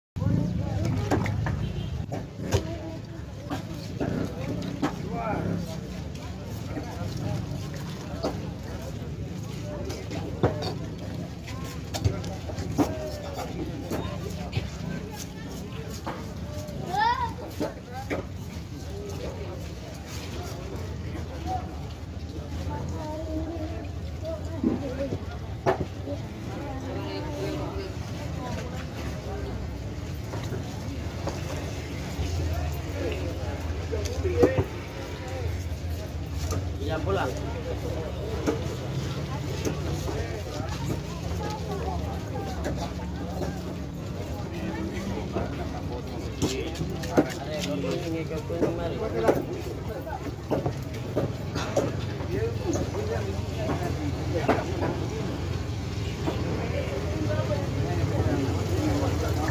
Very Amazing Tuna Cutting Skills
Assalamualaikum warohmatullahi wabarokatuh, selamat datang di Afa Tuna Channel Cara abang Fadil Memotong Ikan dan Skill Golok menakjubkan di Pasar Ikan Jembatan Puri Kota Sorong Papua Barat Indonesia